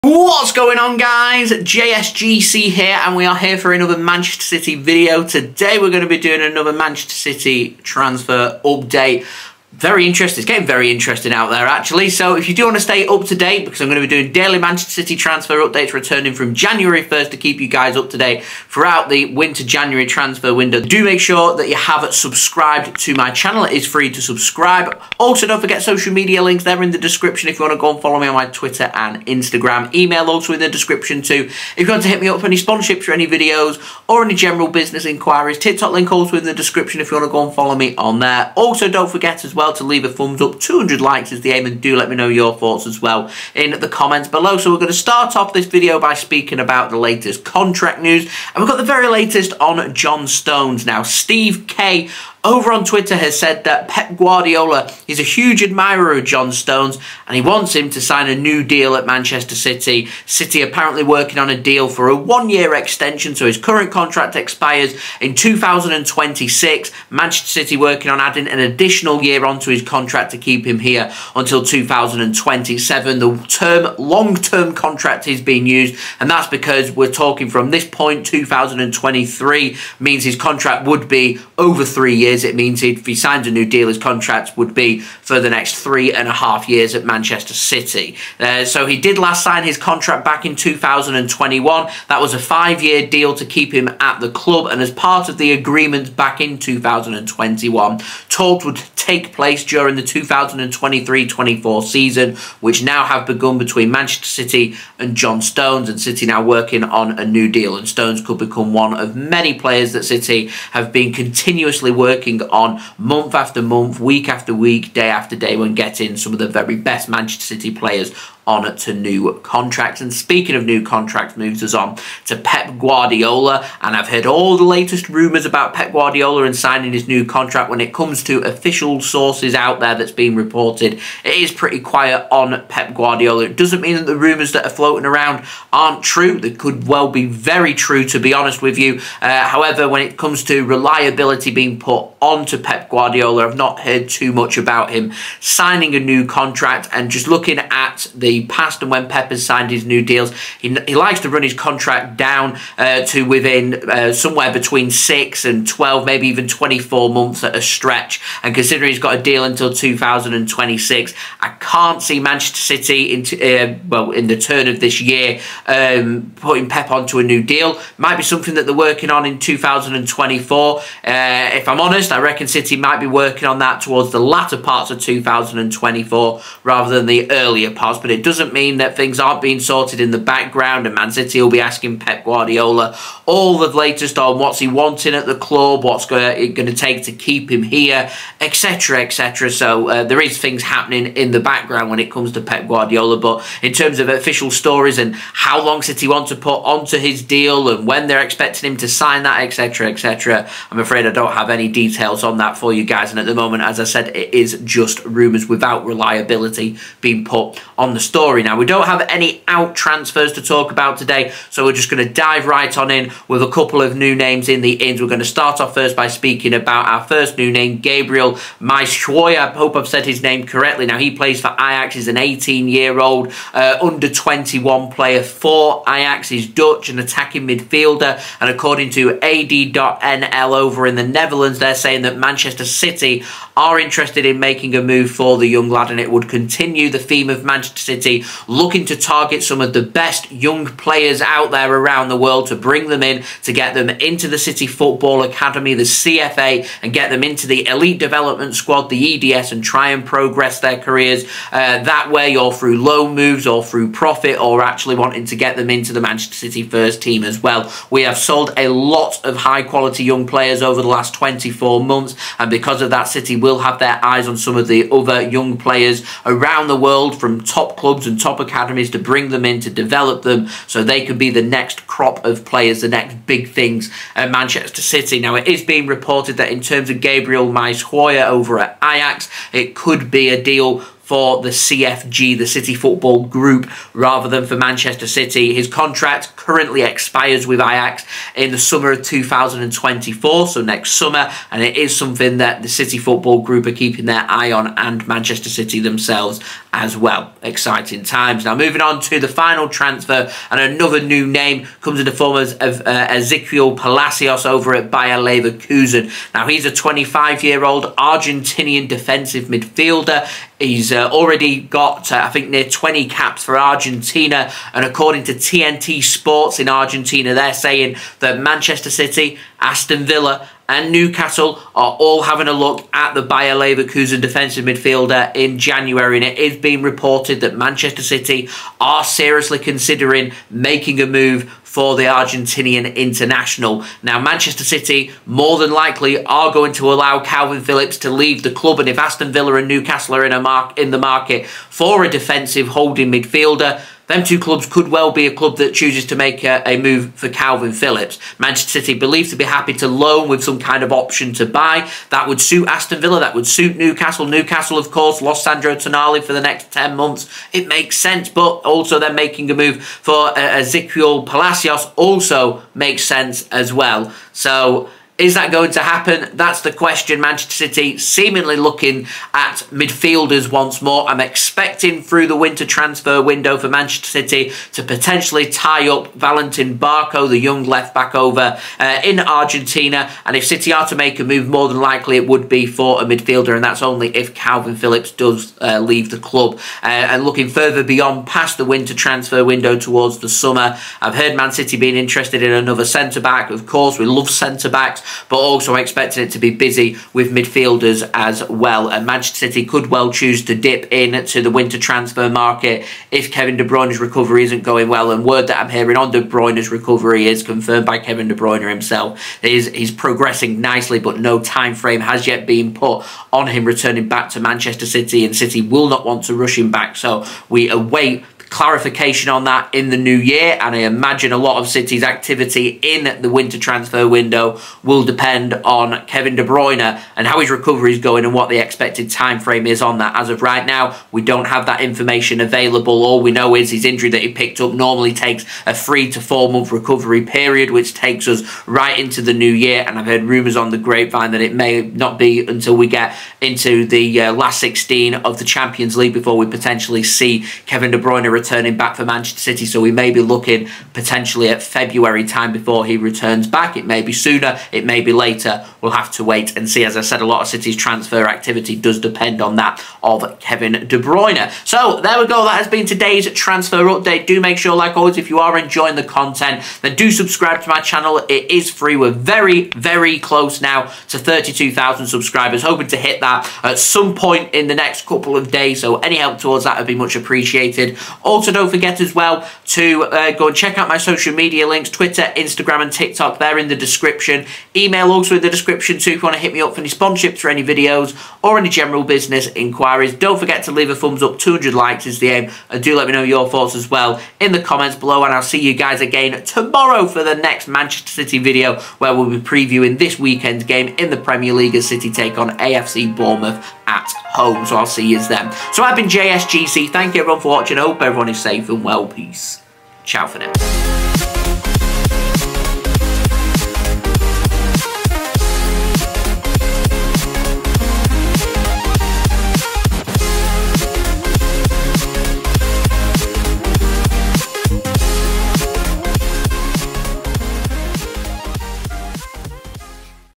Exequiel Palacios In From Bayer Leverkusen, Kalvin Phillips OUT | Man City Transfer Update
Man City transfer update as Man City want John Stones on a new contract, rumours emerge of Pep Guardiola potentially signing a new deal at Manchester City, Ajax youngster Gabriel Misehuoy is linked with a move to both City and the City Football Group (the CFG) and finally, Argentina and Bayer Leverkusen defensive midfielder is linked with a move Man City, Aston Villa and Newcastle United as Man City want Kalvin Phillips to leave this winter. The sources used in this video are: TNT Sports, AD.nl and Steve Kay. Enjoy the video! :-)

Chapters:

0:00 Intro
0:44 Man City want John Stones on a new contract
2:00 Pep Guardiola to sign a new contract at City?
3:29 City want Ajax youngster Gabriel Misehuoy
4:59 Bayer Leverkusen midfielder Exequiel Palacios is on City's radar
7:42 Outro